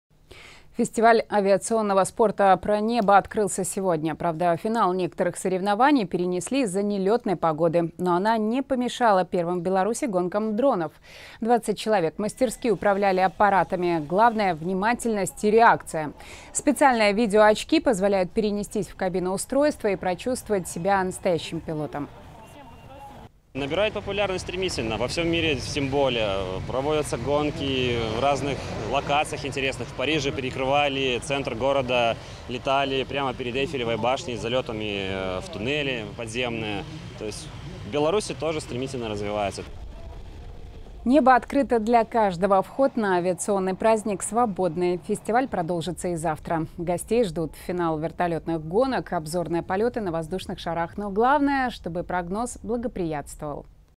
Фестиваль авиационного спорта «ПроНебо» в Минске
Правда, финал некоторых соревнований перенесли из-за нелётной погоды. Но она не помешала первым в Беларуси гонкам дронов.